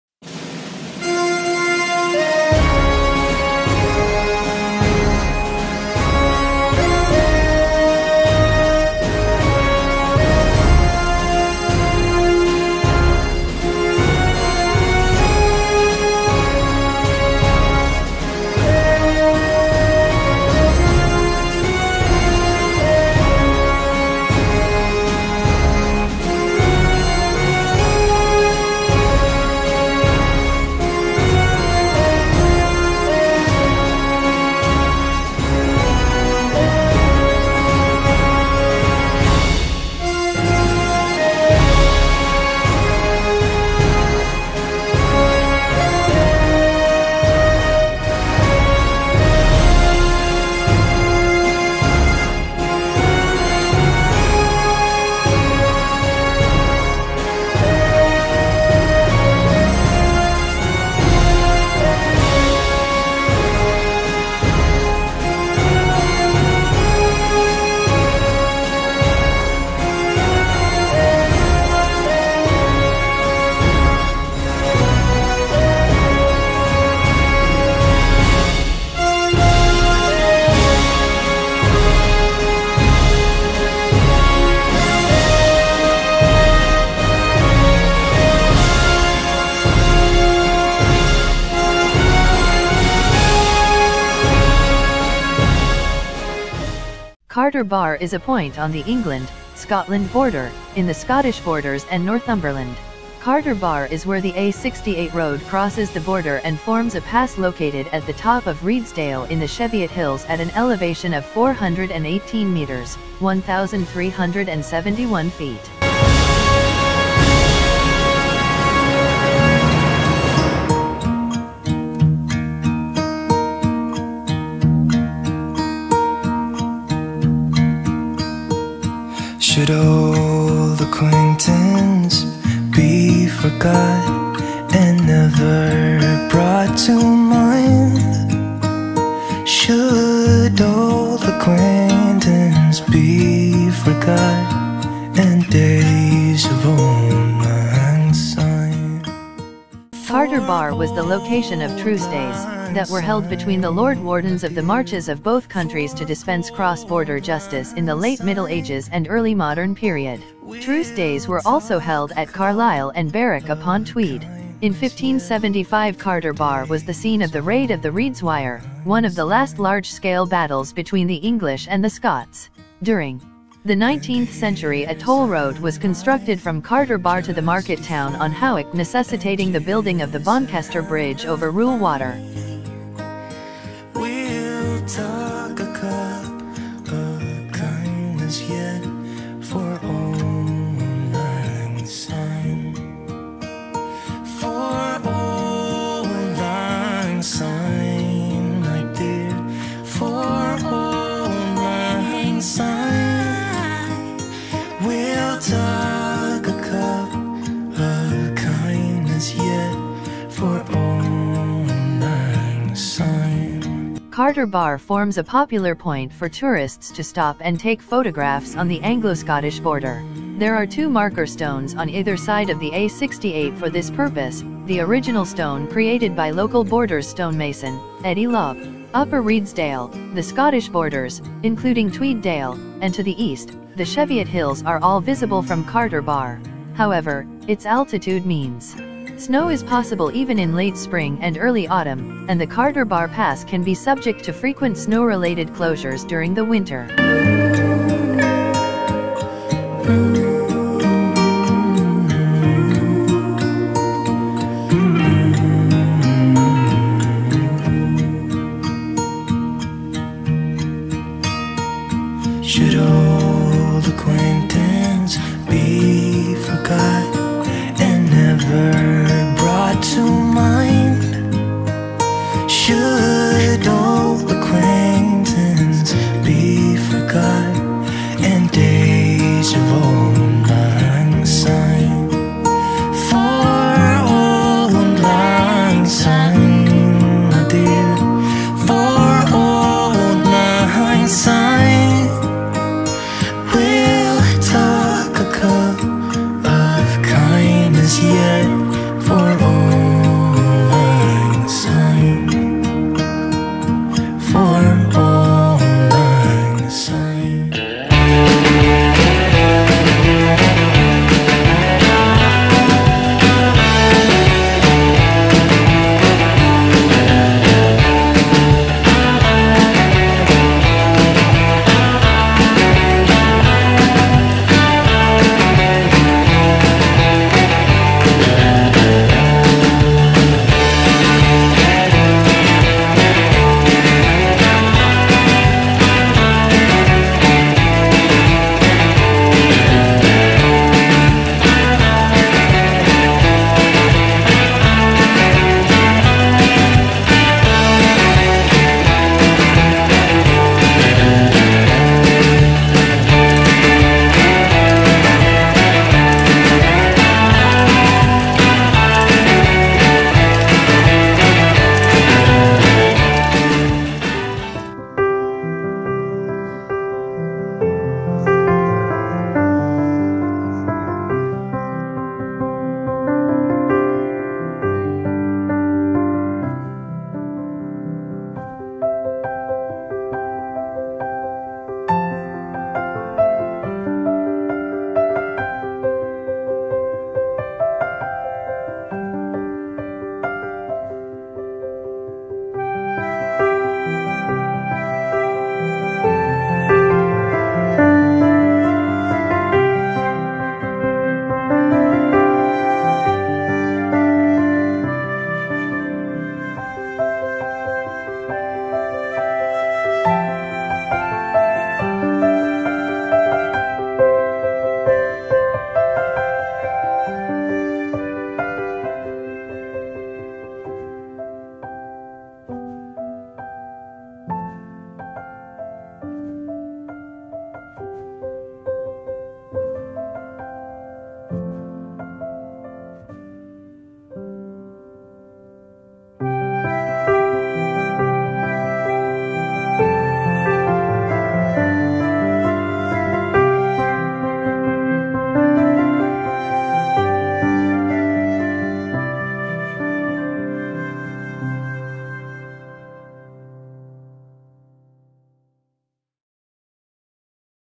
Carter Bar Jedburgh
Quick ride to Carter Bar on my Motorcycle, took some photos and videos of the area using a Nikon P1000 Camera